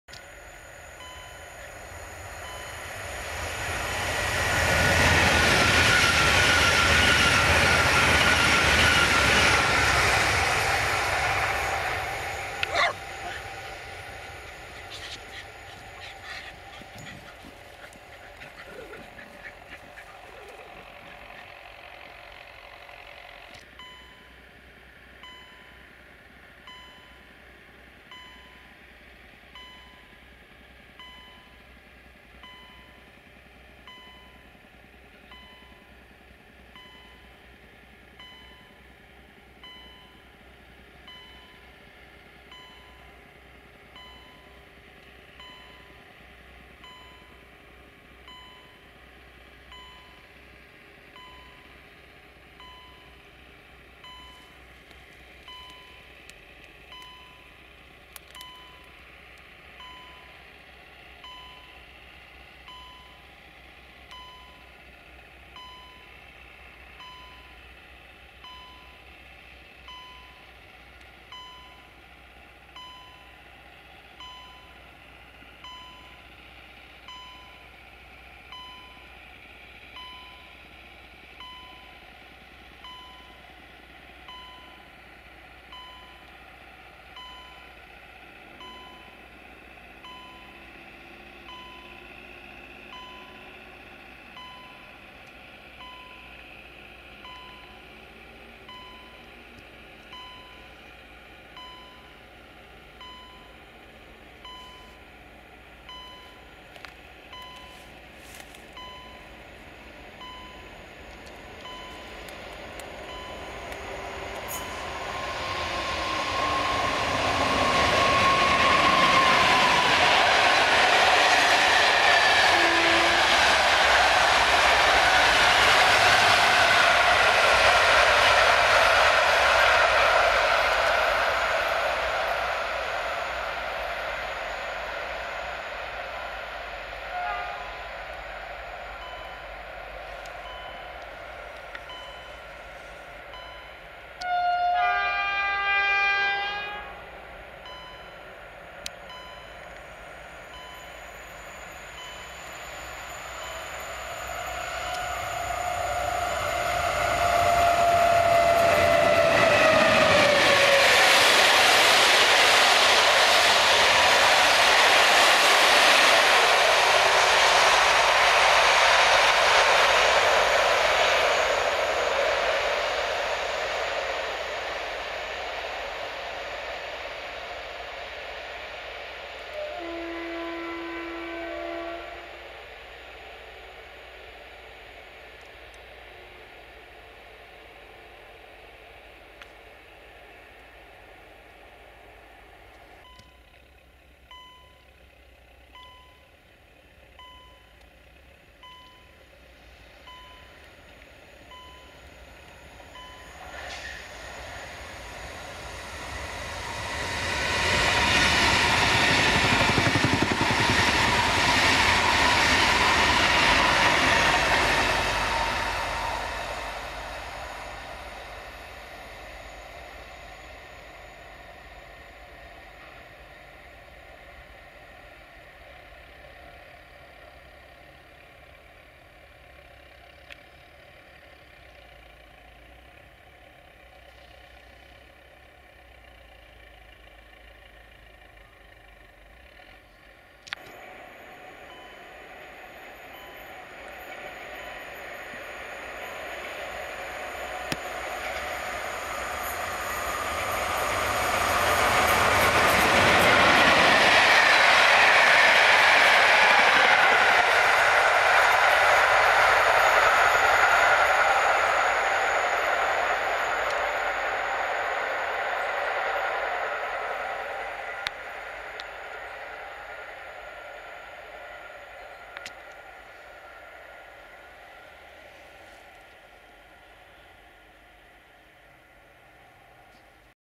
Klimatyczny mix kolejowy
Kilka ujęć w okolicy Kozłowa.
Coś było nie tak z sygnalizacją przejazdową koło Kozłowa bo wszystkie składy zwalniały w tamtym miejscu stąd takie prędkości jak na filmie widać.